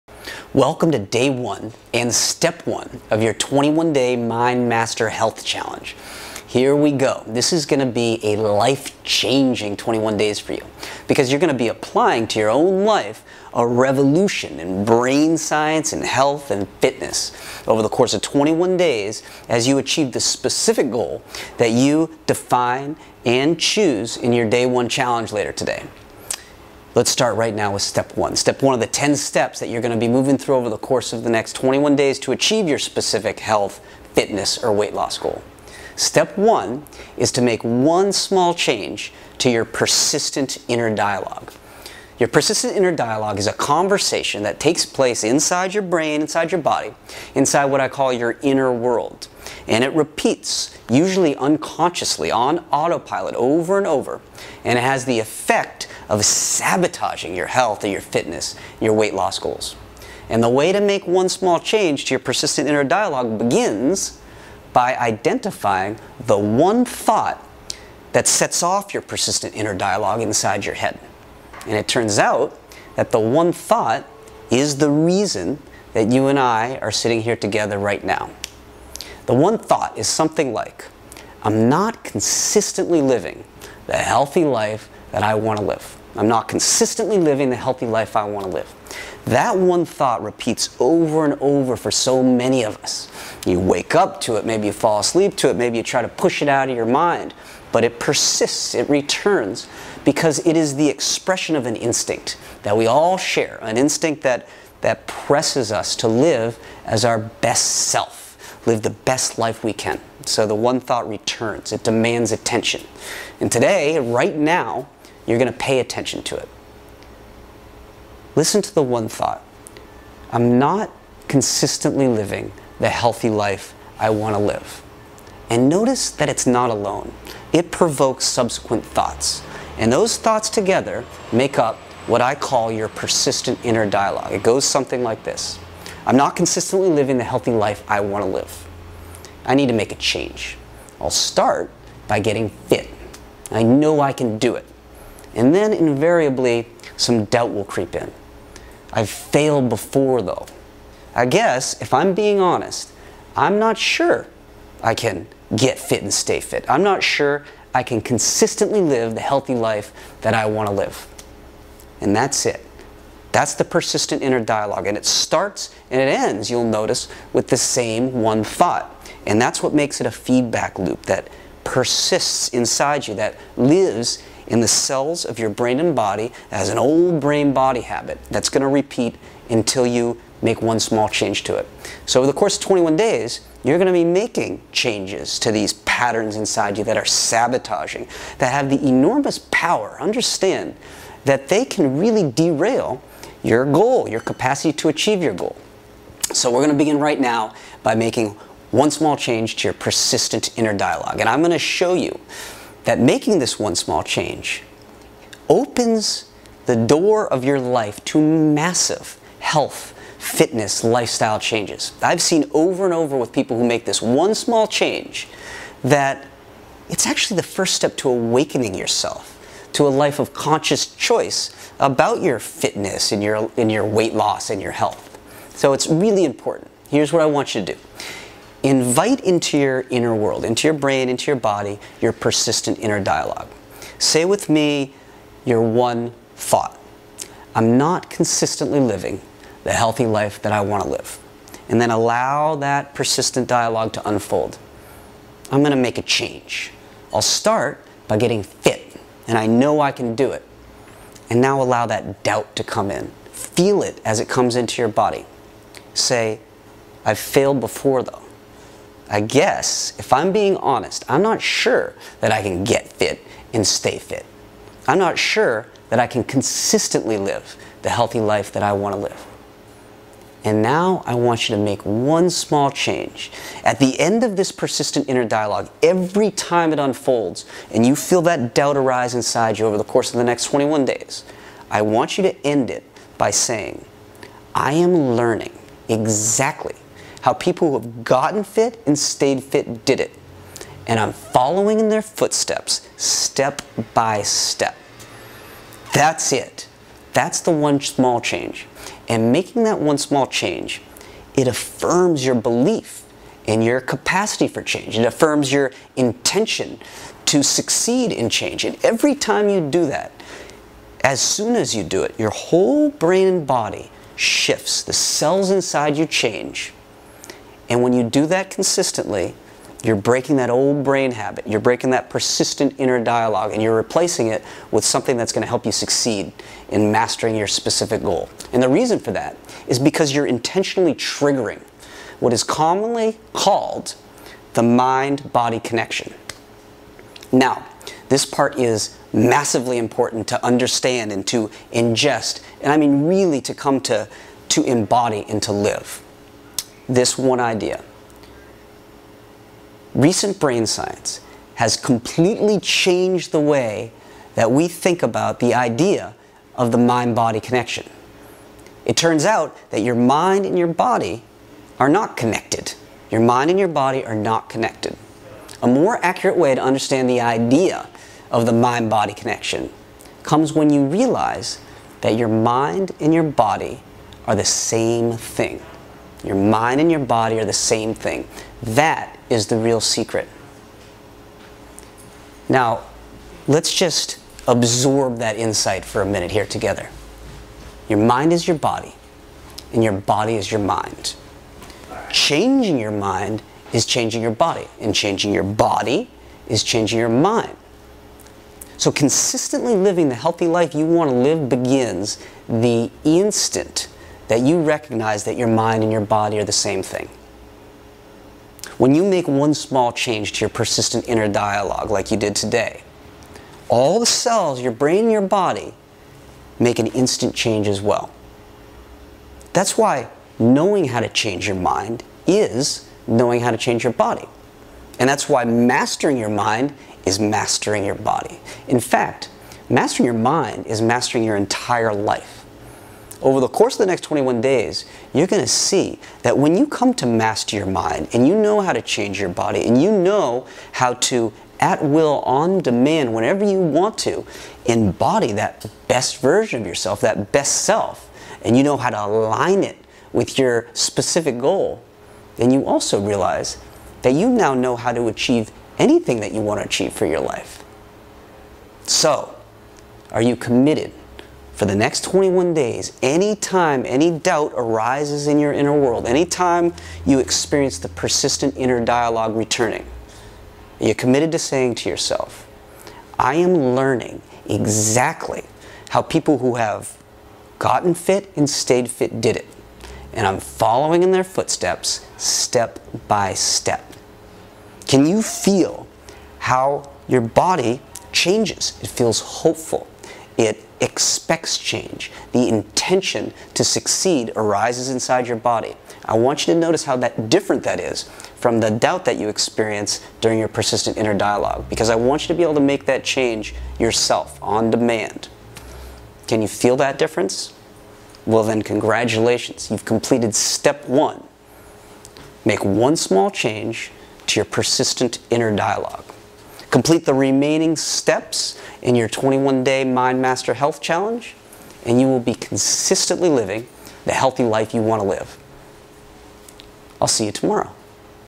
The 21 Day Mind Master Health Challenge: Step 1
Welcome to The 21-Day Mind Master Health Challenge
It takes 21 days for you to rewire your brain with new habits. Dr. Sullivan's Mind Master Program applies the latest brain science to design a unique process for you to blend into your life to rewire your brain and body to achieve your specifically chosen goal, (e.g. master health, master fitness, master weight loss, master stress and anxiety, master happiness), as you create the life you want to live.